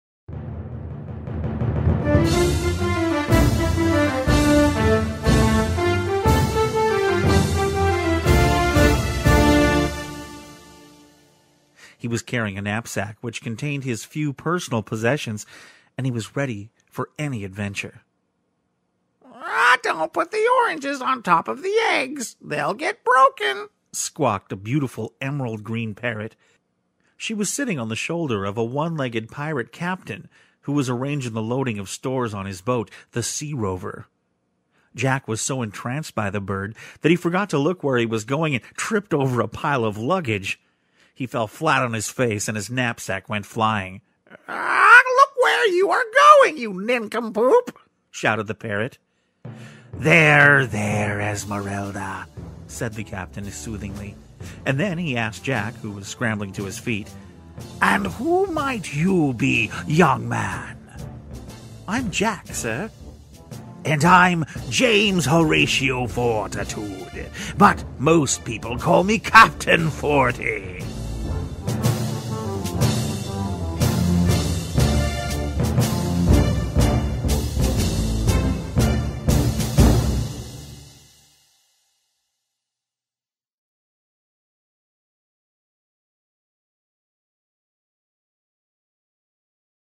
Rick Moyer Storyteller And Music Example
Join me for a quick example of a job I did for the clients I was hired to narrate and do character voices for their Sea Rover adventure.
Would you like me to create for you?  Hire me today!